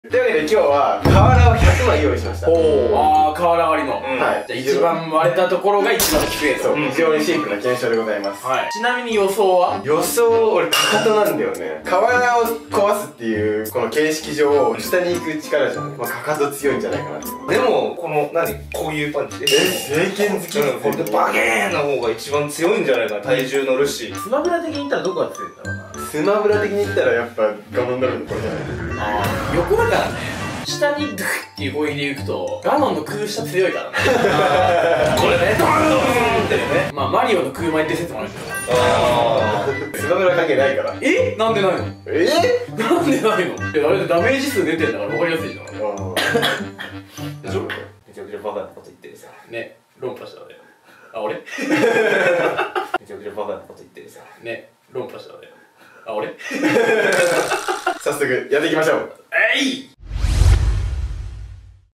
【陸上トリオ】"例え"でりょうを論破したと勘違いしてるてつやの流れが好きすぎるwww【東海オンエア】
↓↓今回使用させて頂いた動画は↓↓

【喧嘩必見】身体のどこで攻撃すると一番つええの？